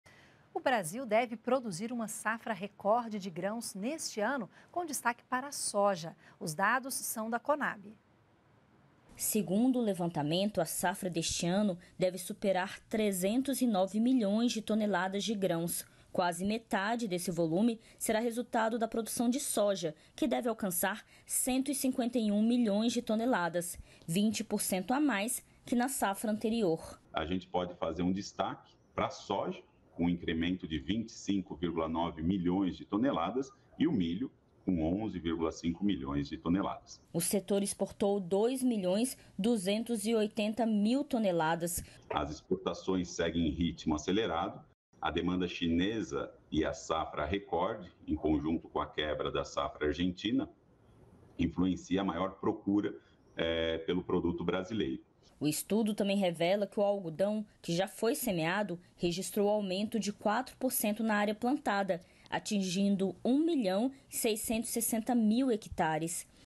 Agricultura | Safra recorde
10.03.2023 - Brasil deve ter safra recorde e colher 310 milhões de toneladas de grãos.